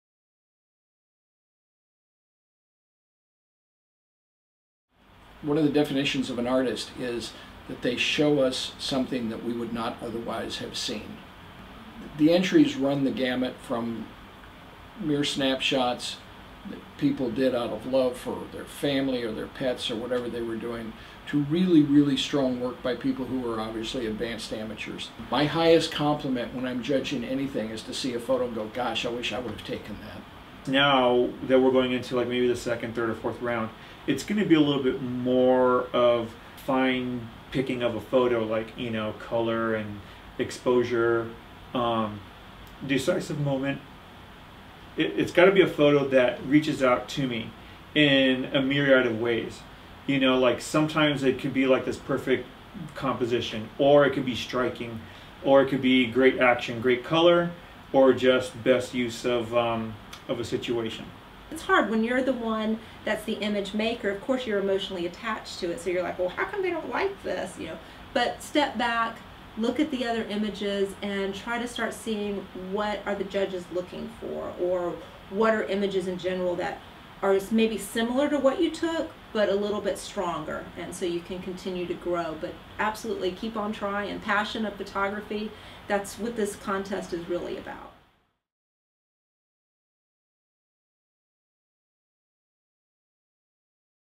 2016 Buzz Photo Contest Judging
Ever wondered about what goes on behind the scenes of the Buzz Photo Contest judging? The 2016 Buzz Photo Contest judges - Michael Hart, Nick De La Torre and Nikky LaWell - talk about what makes a great photo. See the winners in our July 2016 issue. (Video: Dylan Aguilar)